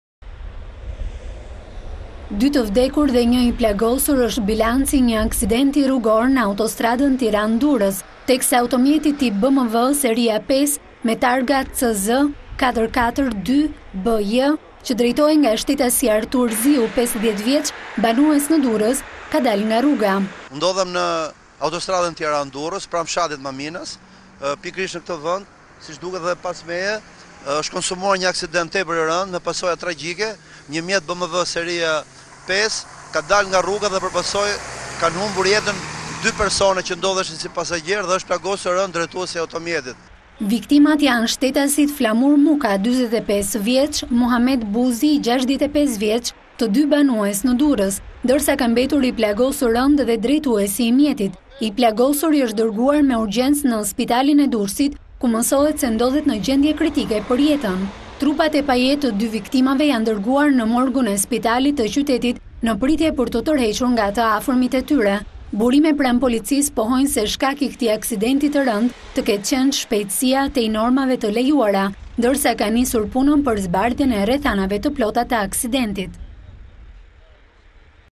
A1 Report - Tiranë-Durrës, makina del nga rruga, 2 të vdekur, 1 i plagosur
DURRËS - Tiranë- Durrës, teksa automjeti tip "BMË seria 5", me targa CZ 442B J, që drejtohej nga shtetasi Artur Ziu, 50 vjeç, banues në Durrës, ka dalë nga rruga. Për pasojë kanë gjetur vdekjen shtetasit, Flamur Muka, 45 vjeç, dhe Muhamed Buzi, 65 vjeç, ndërsa ka mbetur i plagosur rëndë edhe drejtuesi i mjetit. I plagosuri është dërguar me urgjencë në spitalin e Durrësit, ku mësohet se ndodhet në gjendje kritike për jetën. Burime pranë policisë pohojnë se shkak i këtij aksidenti të rëndë të ketë qenë shpejtësia tej normave të lejuara, ndërsa uniformat blu të Durrësit kanë nisur punën për zbardhjen e rrethanave të plota të aksidentit.